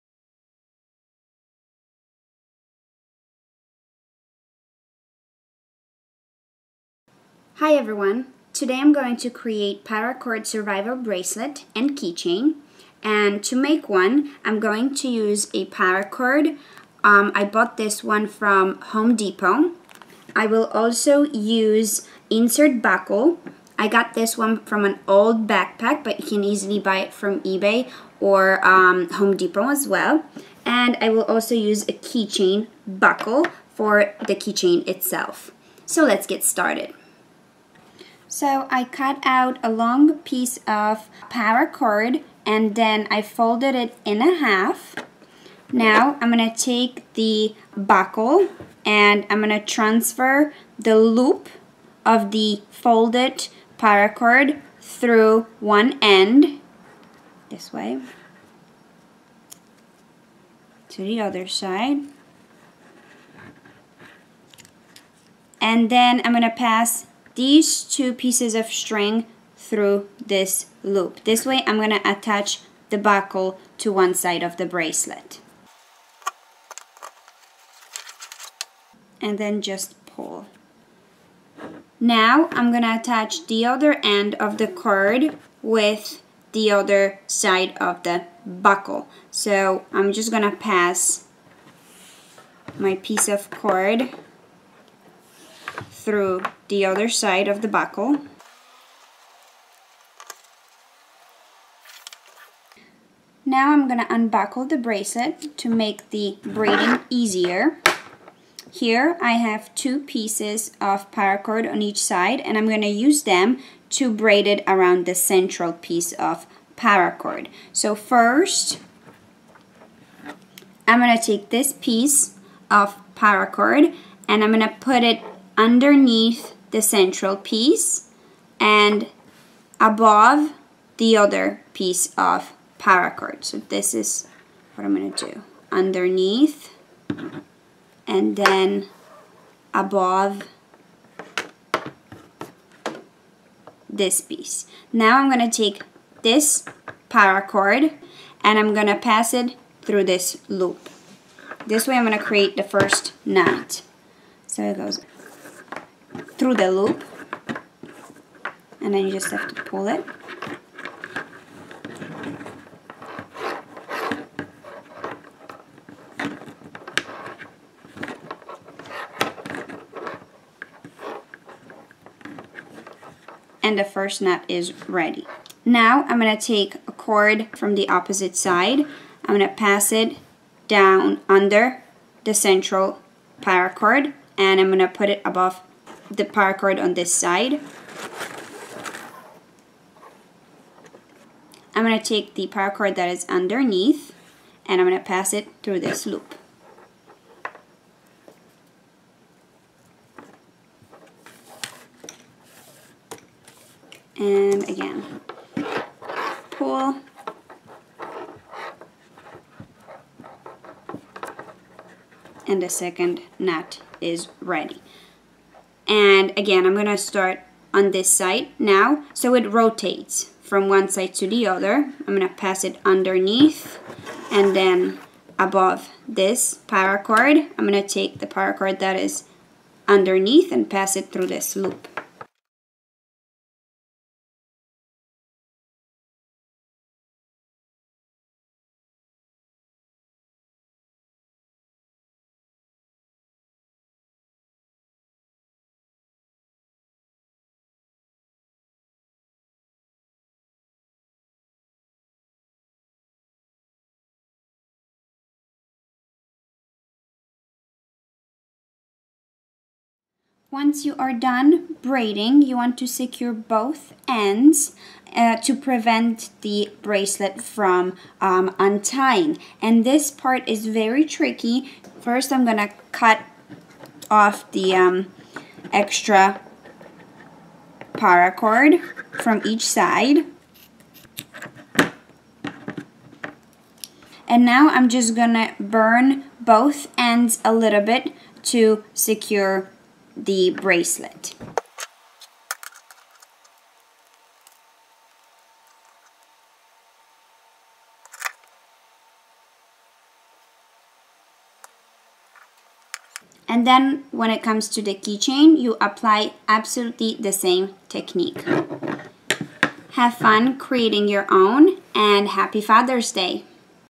DIY Paracord Survival Bracelet - Father's Day Gift Idea
Paracord Survival Bracelet is an excellent DIY gift for Father's Day. Here I am going to show you how to make your own survival bracelet and key chain.